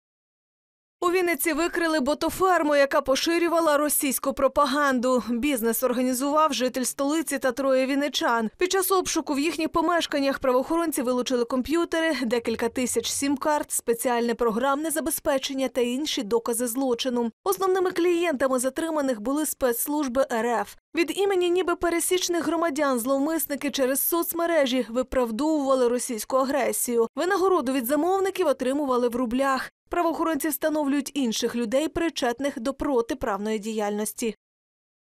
У Вінниці викрили ботоферму, що пропагувала інтереси кремля
Ботоферму, яка поширювала російську пропаганду викрили у Вінниці. Її організували житель Києва та декілька вінничан. У їхніх помешканнях правоохоронці вилучили речові докази злочину. Основними клієнтами затриманих були спецслужби рф. Зловмисники виправдовували війну в Україні через соцмережі. Винагороду від замовників отримували в рублях.